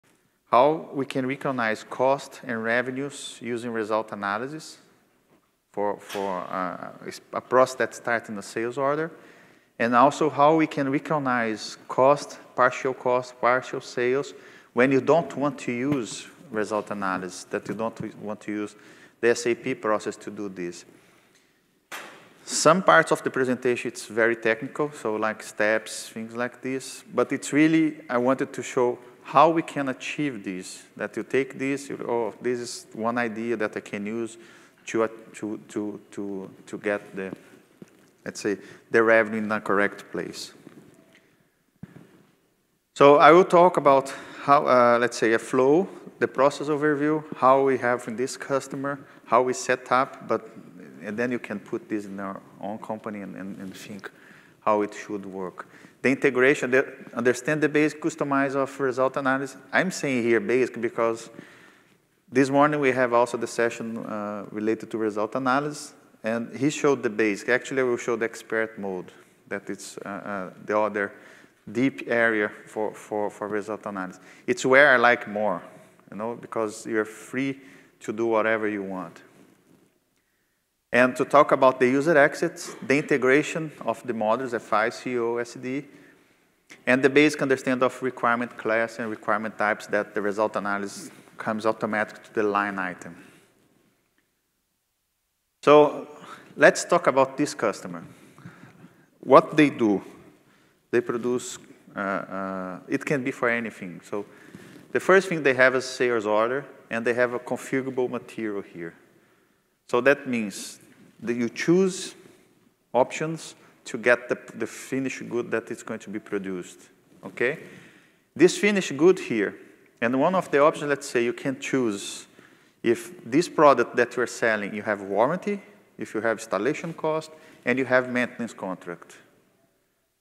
2017 SAP Controlling: Leverage Product Cost by Sales Order
Many companies recognize revenues while a product is built until it is delivered. This ECC-focused session will explore how to use product cost by sales order to recognize revenues an costs in sales contracts using SAP Results Analysis across different periods and different drives for revenue and cost recognition.